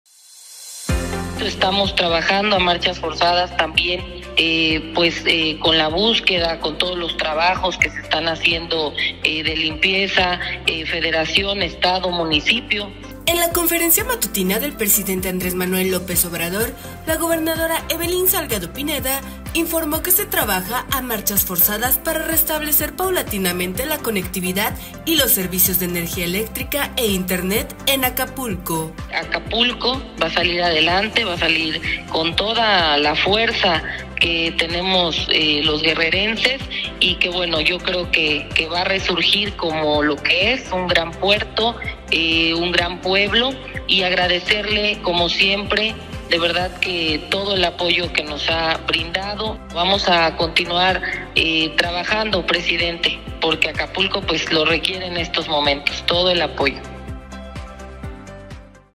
Se trabaja a marchas forzadas para restablecer paulatinamente la conectividad y los servicios
En la conferencia matutina del Presidente Andrés Manuel López Obrador, la gobernadora Evelyn Salgado Pineda informó que se trabaja a marchas forzadas para restablecer paulatinamente la conectividad y los servicios de energía eléctrica e internet en Acapulco.